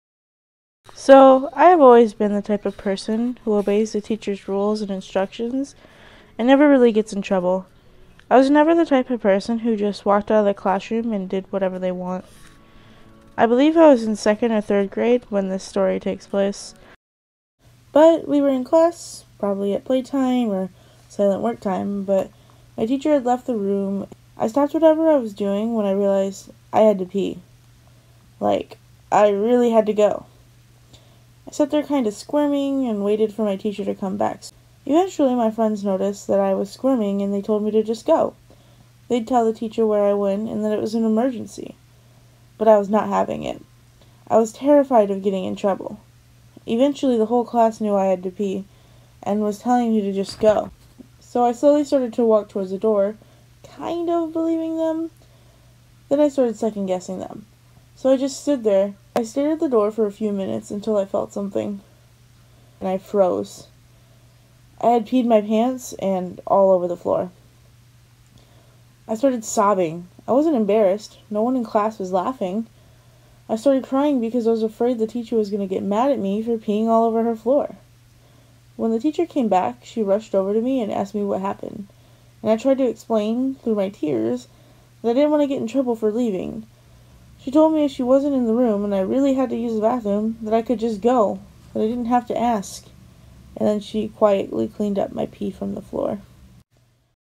Peeing My Pants In the Middle Of Class
A little storytime animation of the time I peed my pants in 2nd grade. I hope to keep uploading and getting better.